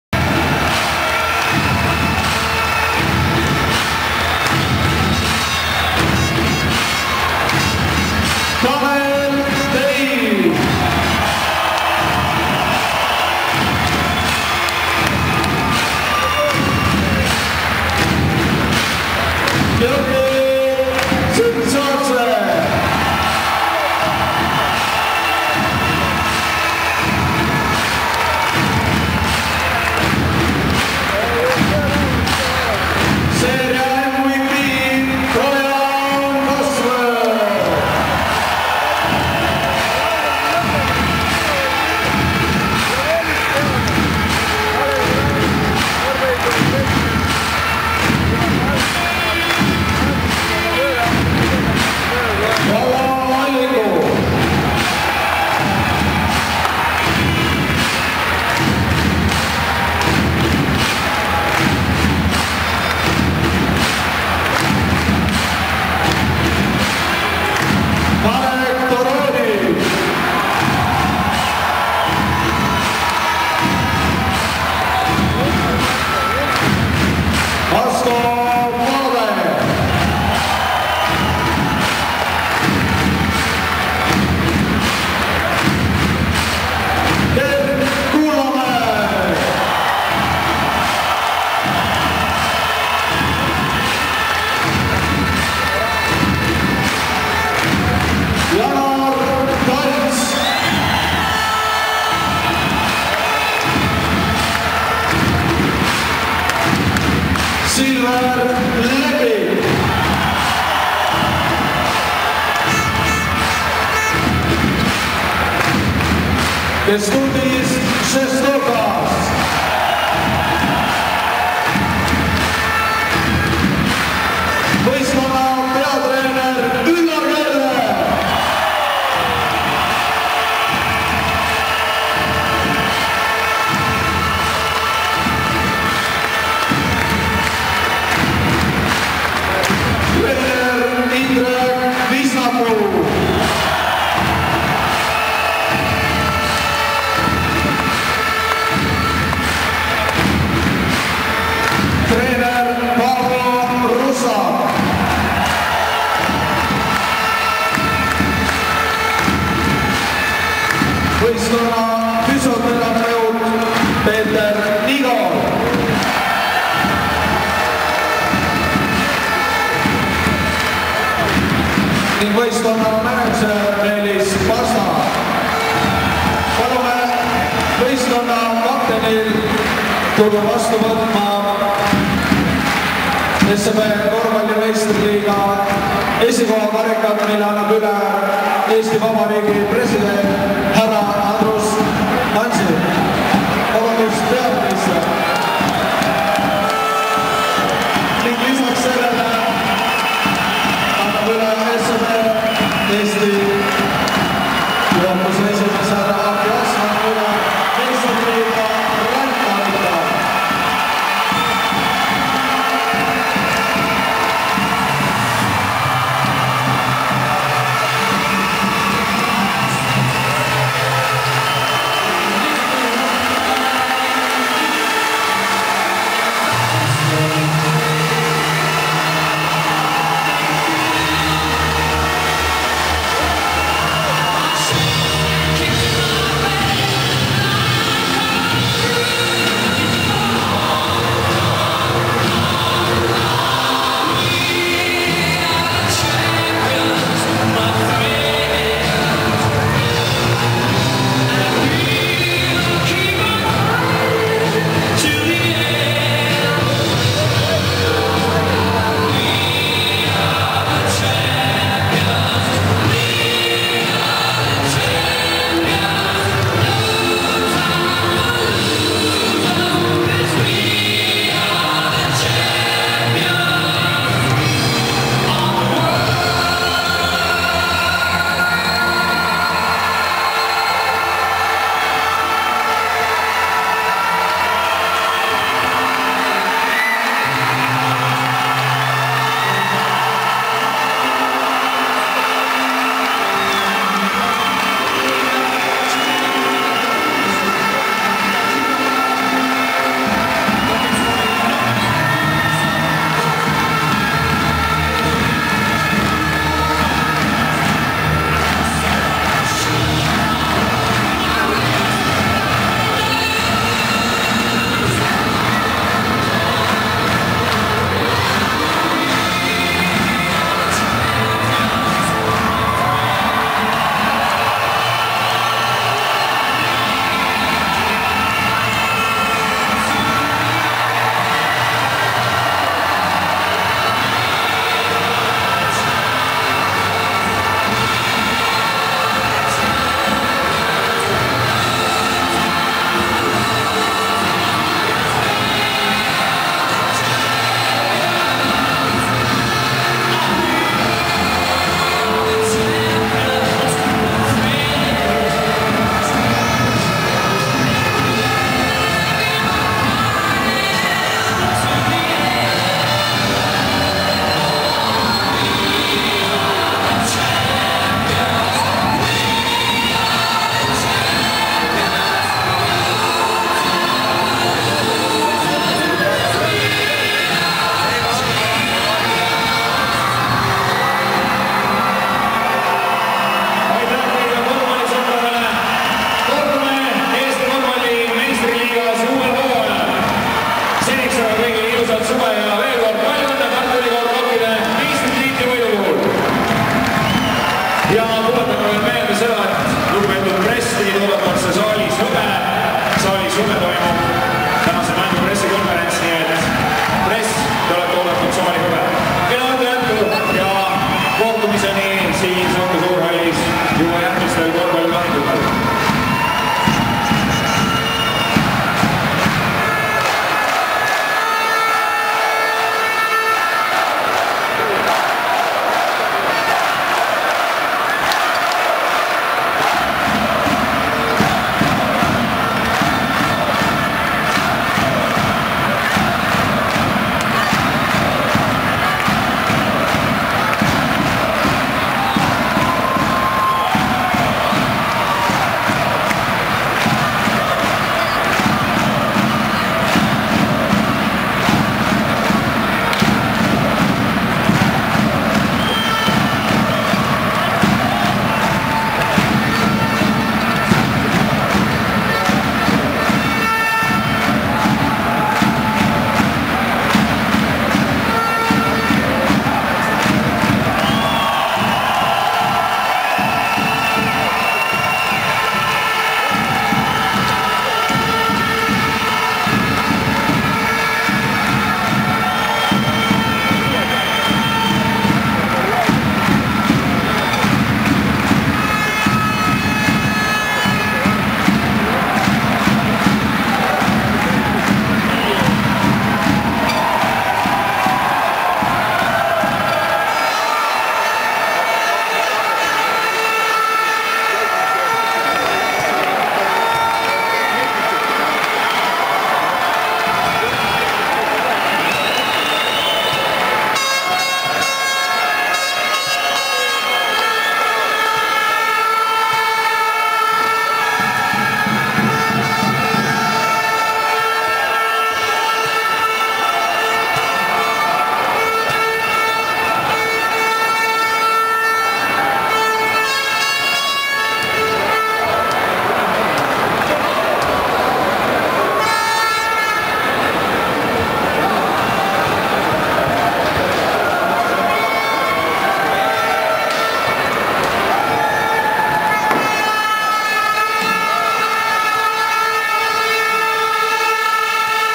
2008 a Eesti meister korvpallis Tartu Ülikool Rock
Tartu Ülikool Rock tuli 22 punktisest kaotusseisust välja ja võitis meistritiitli,
alistades Tallinna Kalev Cramo üldseisuga 4-0.